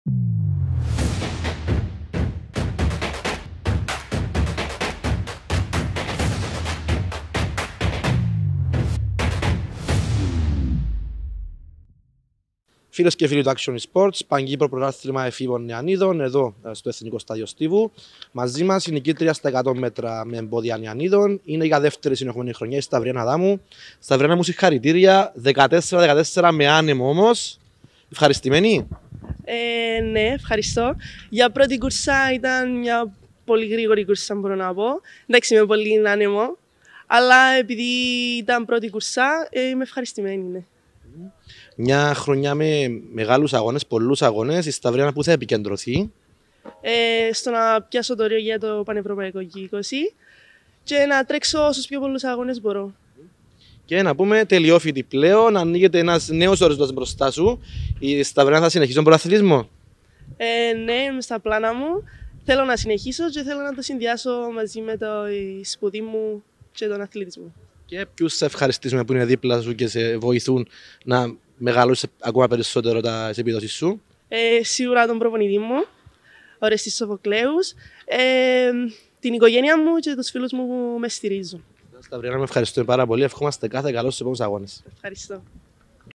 Σταυριάνα Αδάμου: Χρυσό μετάλλιο και στόχος το όριο για το Πανευρωπαϊκο στο Ισραήλ
Welcome to the official ACTION IN SPORTS YouTube channel.
Find us on the internet:
Production by Action IN Sports
Reporter Paris J Andreou and Stelios Stylianou